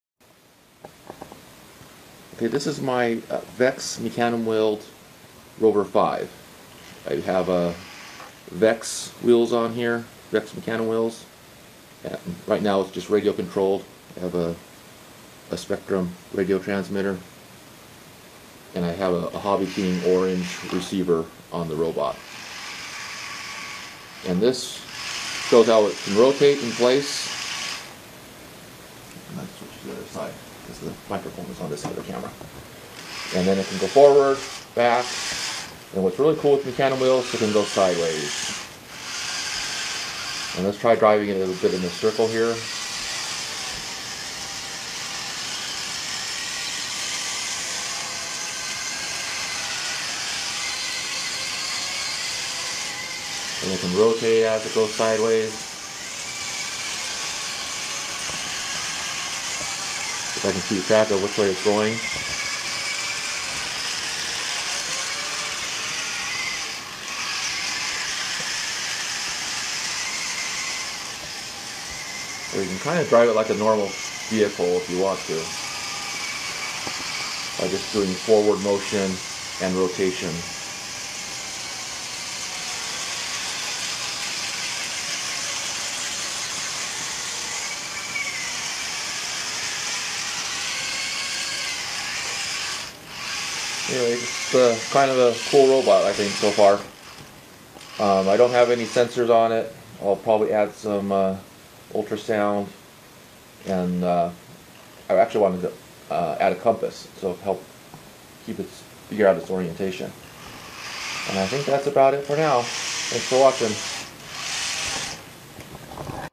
4WD Mecanum wheel mobile Arduino robot kit
wholesale 4WD Mecanum wheel mobile Arduino robot kit at low price in China!

This 4WD drive Mecanum wheel platform vehicle is stable and can be made to move in any direction and turn by varying the direction and speed of each wheel. Includes 100mm Aluminium Mecanum wheels, Faulhaber 12V motors with optical encoders, Arduino 328 Controller, Arduino IO Expansion, ultrasonic and IR sensors.